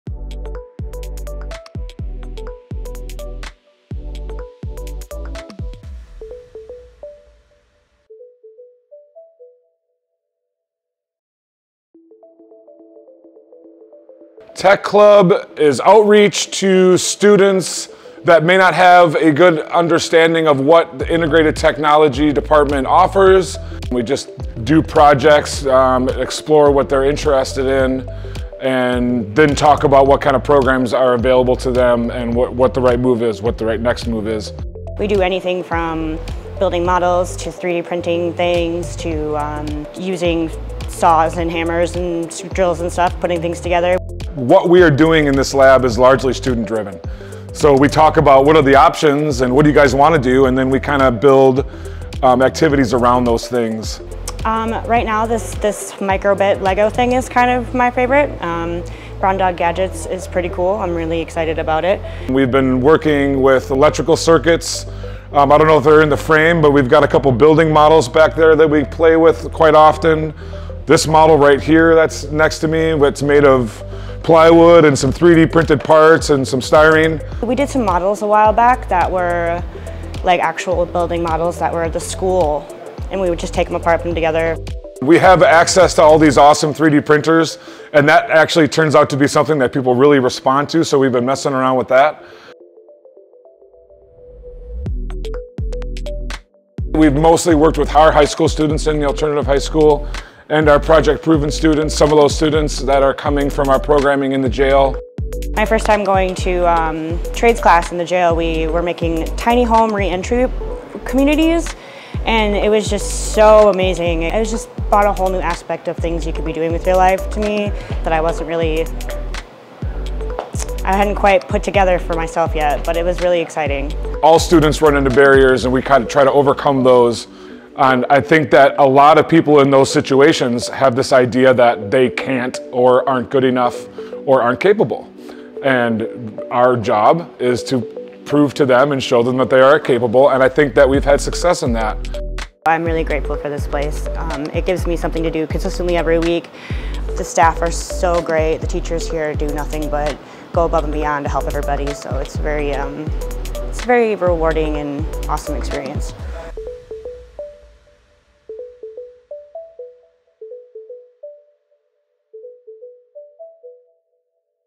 Tech Club at Western Technical College
Engage in hands-on activities to gain an understanding of technical occupations. Discover school options, career pathways, new talents, and more. Explore woodworking, 3D printing, laser engraving and cutting, soldering, mechanical tools, and design software.